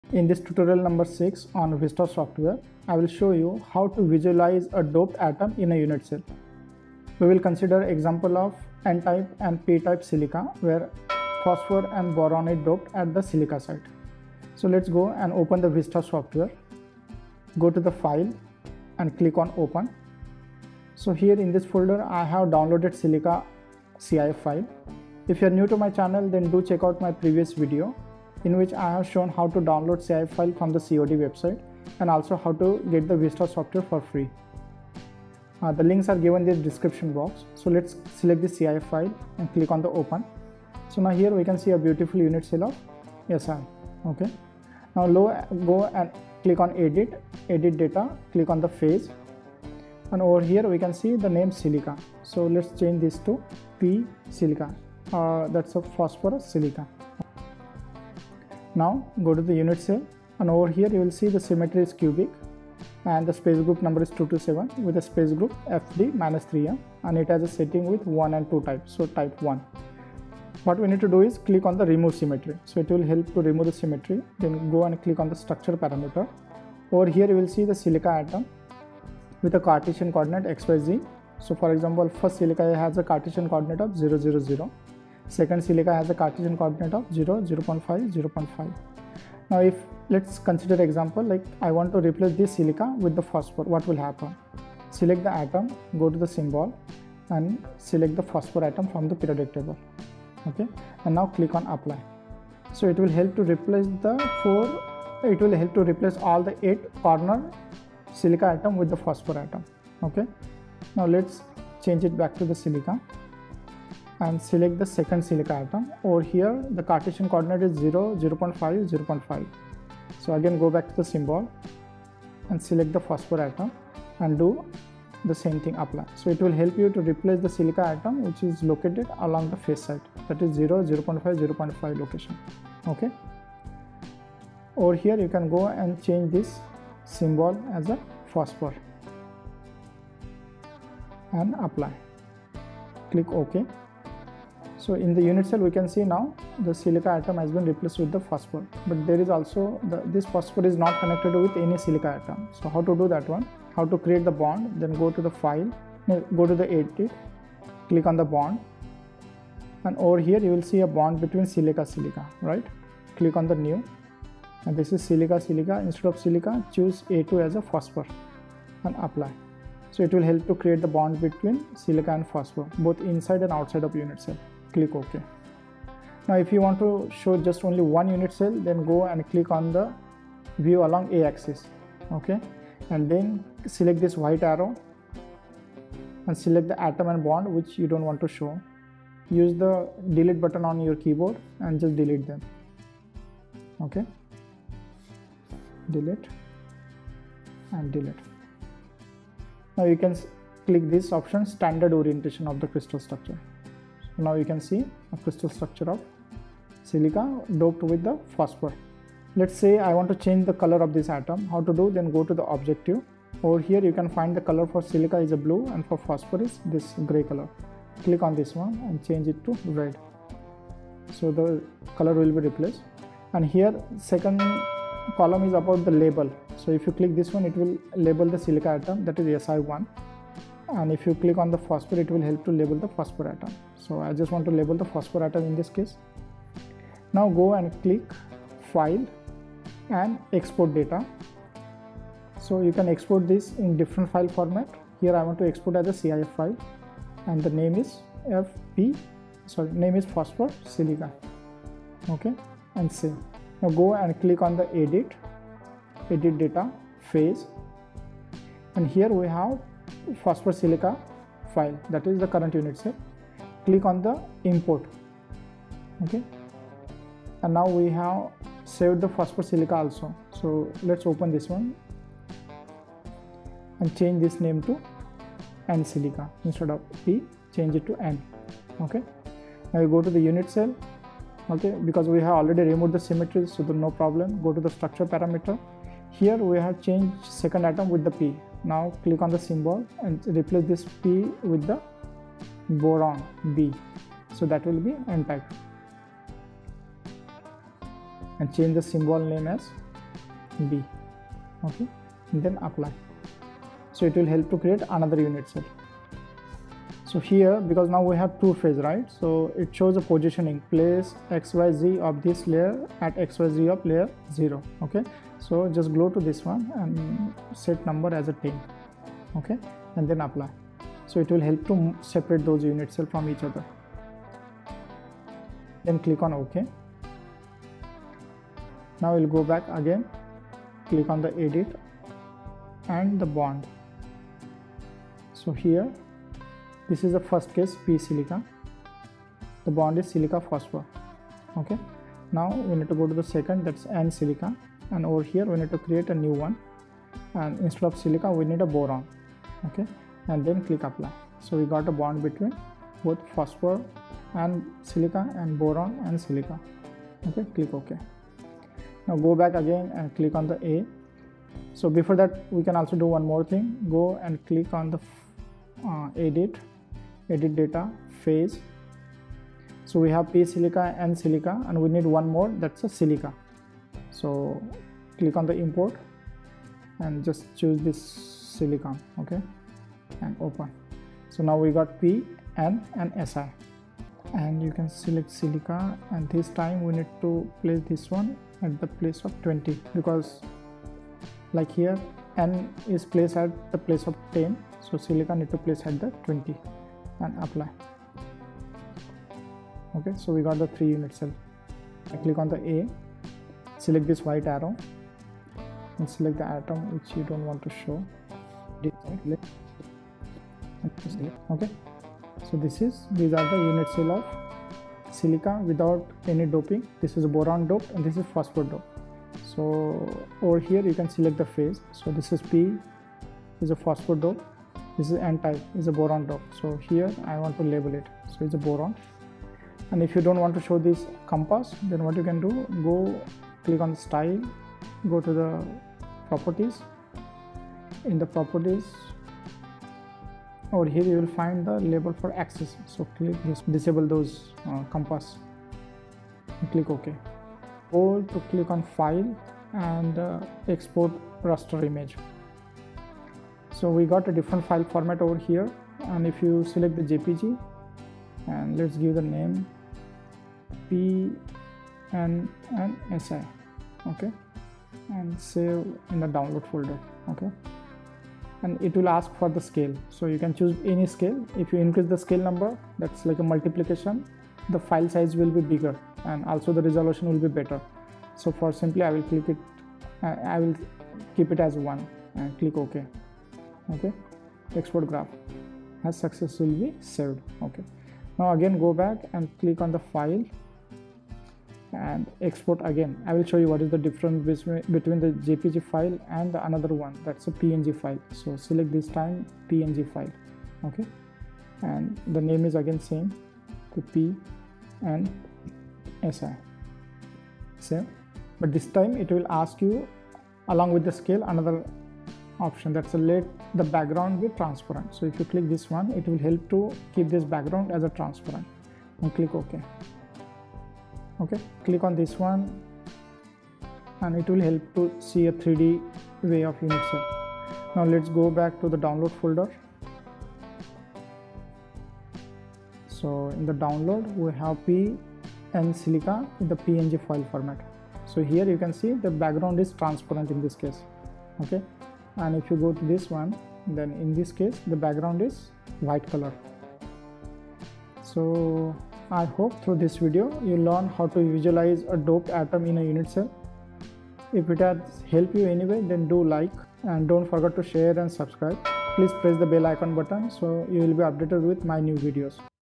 Create n- and p-type doped semiconducting Si unit cell with VESTA Tutorial 6
In this short Tutorial, learn to visualize doped atoms in a unit cell with VESTA software. I have shown examples of doping phosphorus (P) and Boron (B) in the unit cell of semiconducting silicon (Si) unit cell. Oven the VESTA software, click on the file and open the cif file of Si. Then go to edit and click on phase. Here, you have to remove the symmetry and then replace one of the Si atoms with P or B and click on apply. Then go to edit and add the bond between Si and doped atom and click on apply. Then, change the color of the atoms and export the file as a raster image in either JPEG or PNG file format. Follow the step-by-step tutorial for further details.